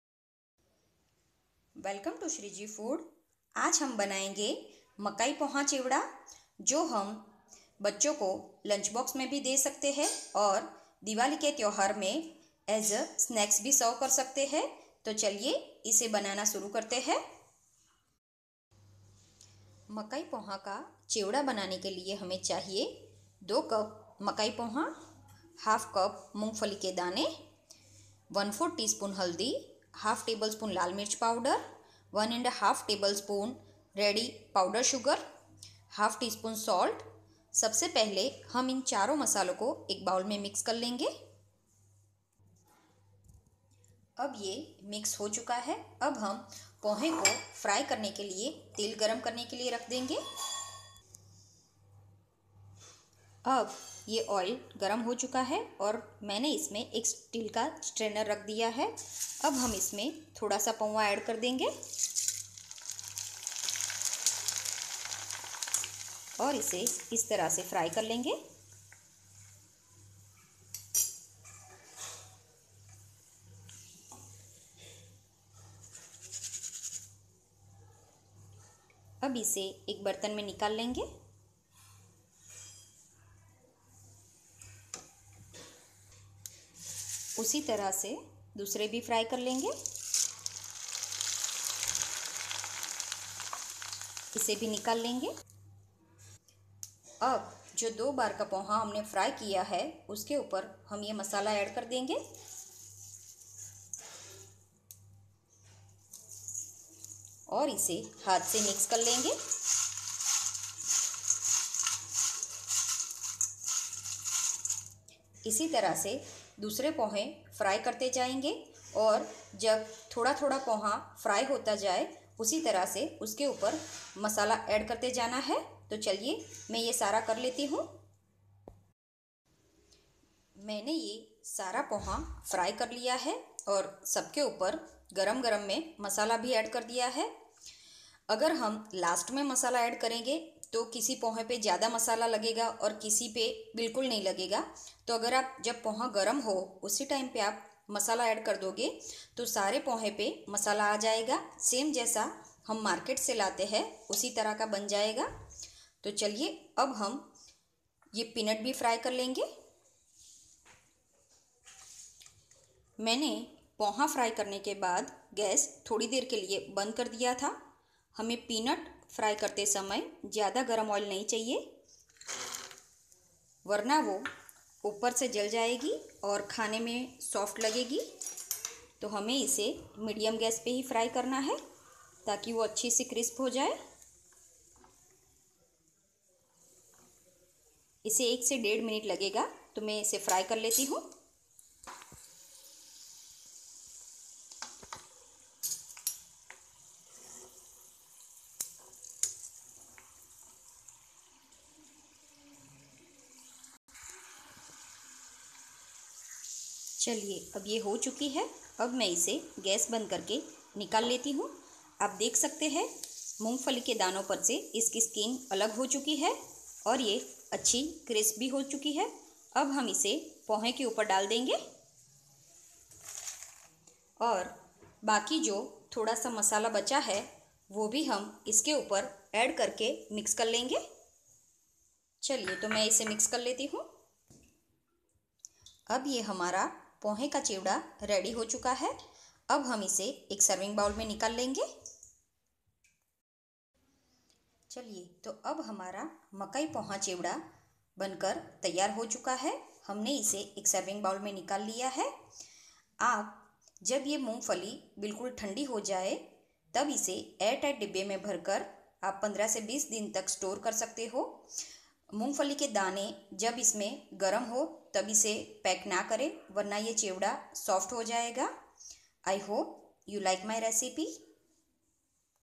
मकाइ पोहा का चेवडा | Cornflakes Chivda | Makai Poha Chivda Recipe
Cornflakes chivda is very easy and tasty recipe.we can serve this chivda as a dry snacks on diwali and also give this in kids lunch box.
2 c cornflakes
1/2 c peanut
1/4 ts termric powder
1/2 tb red chilli powder
1-1/2 tb powder sugar
1/2 ts salt
Oil for frying
FOR GUJARATI VIDEO